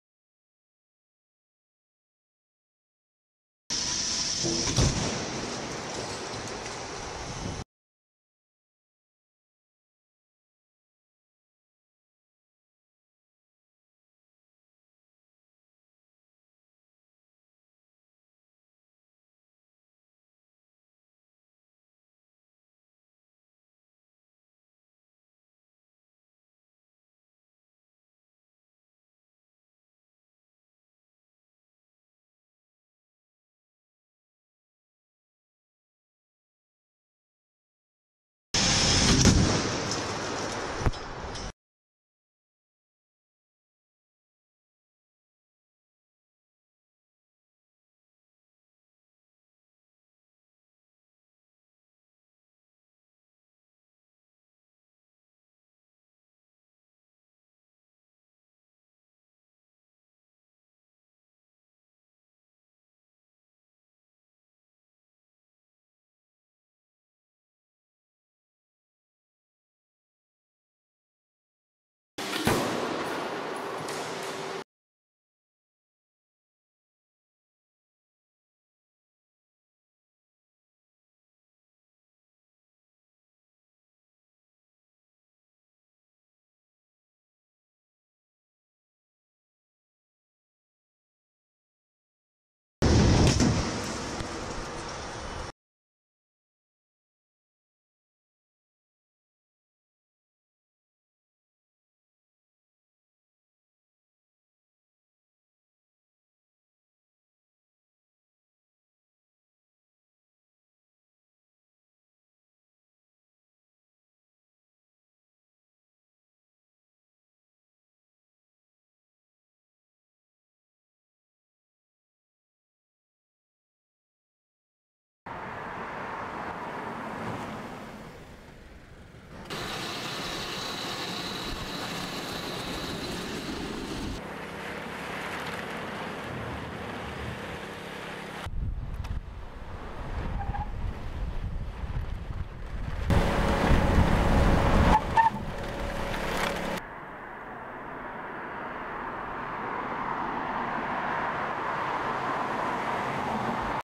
La Subaru XV obtient cinq étoiles aux crash-tests Euro NCAP
La Subaru XV obtient cinq étoiles à l'issue des crash-tests de sécurité passive effectués par l'organisme indépendant de sécurité Euro NCAP 2017.